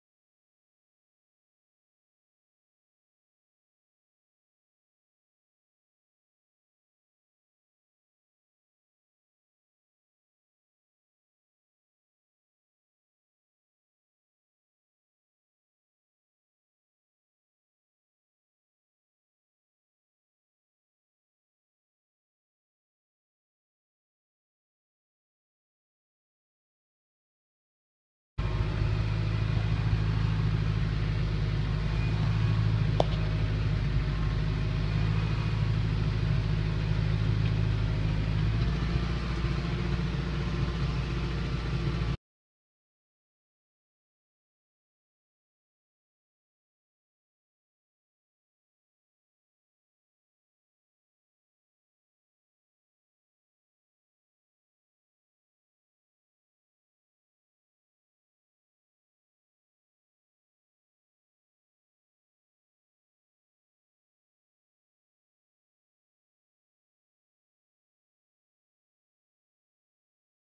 Firewood operations with a tractor, logging winch and firewood boxes..
Using a firewood box and a logging winch on a tractor to extract firewood. It discribes the operations and pros and cons of this system.